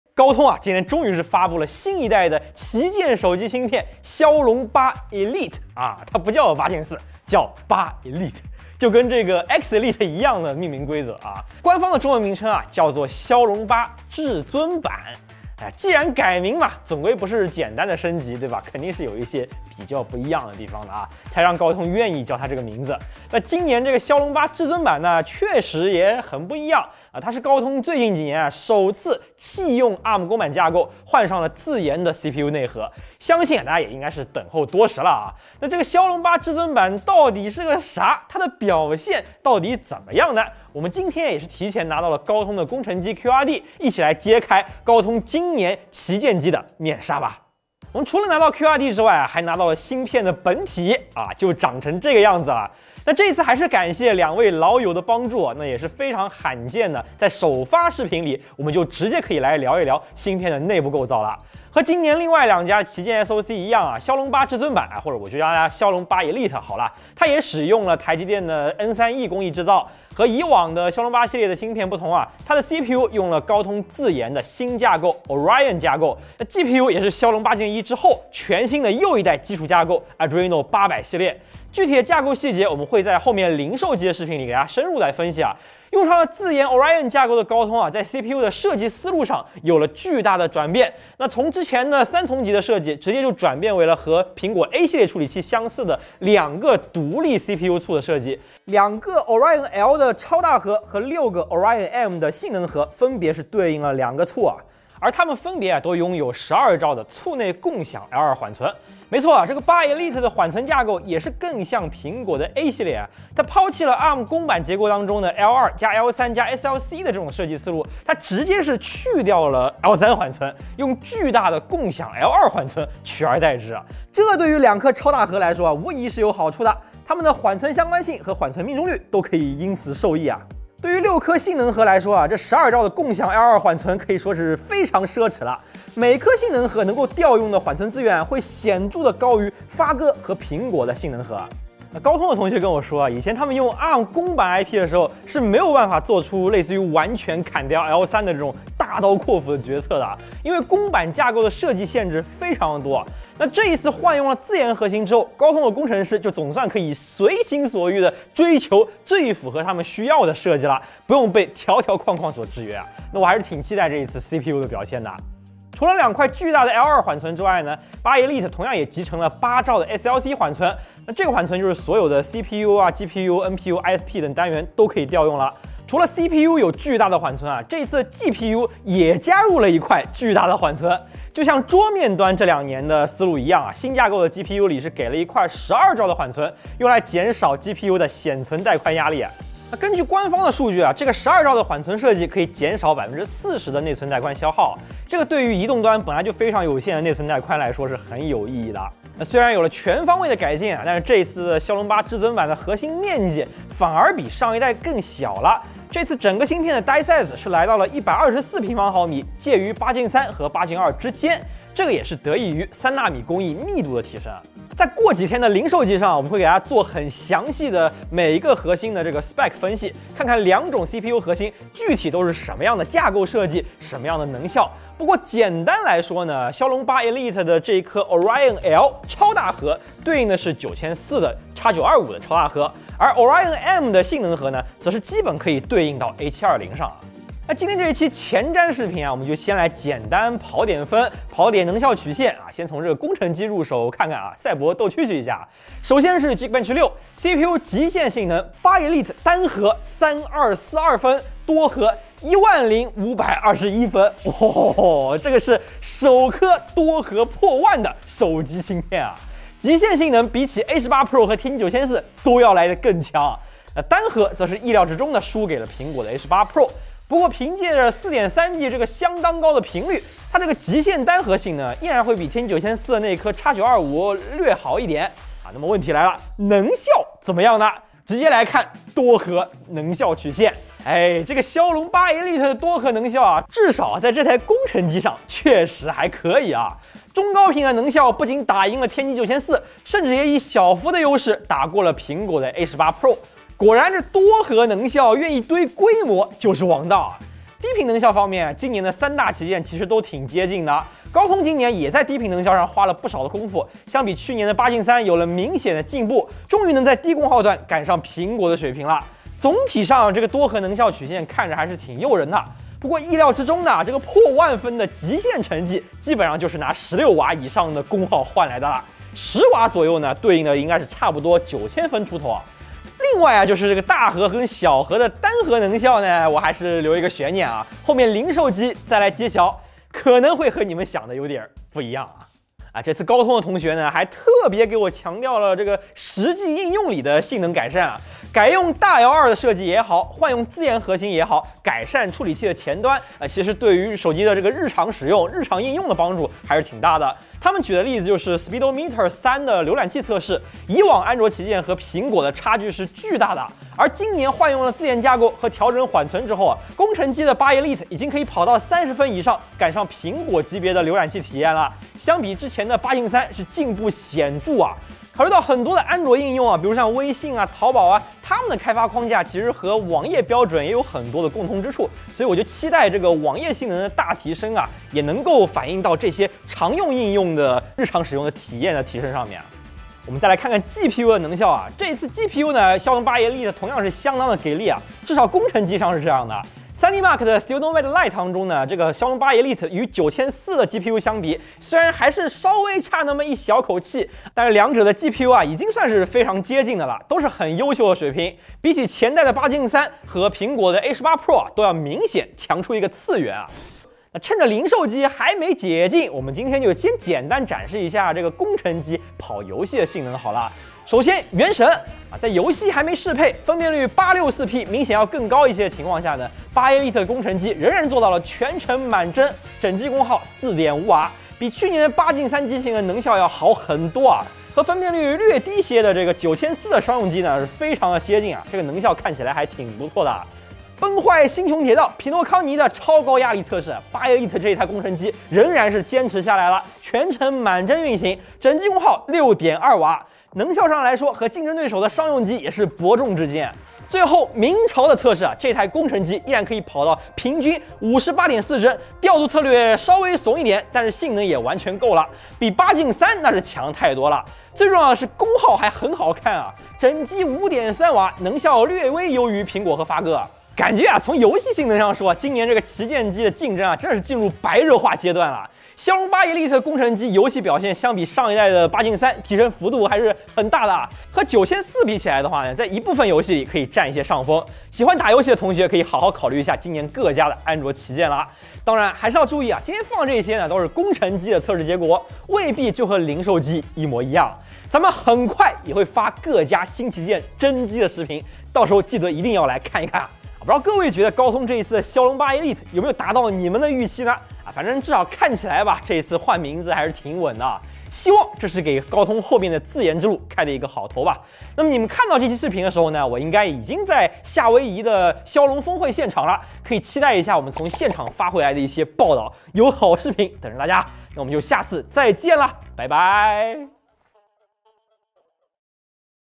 高通新旗舰来啦！骁龙8至尊版强不强？
大家久等了！今天高通终于发布了新一代旗舰手机芯片——骁龙8 Elite，中文名骁龙8至尊版。高通每每改名总是伴随着一些大事发生，那这次骁龙8 Elite自然不例外，作为高通近年来第一款自研CPU的产品，他到底是什么规格？能效有看头吗？我们先拿工程机前瞻一下吧……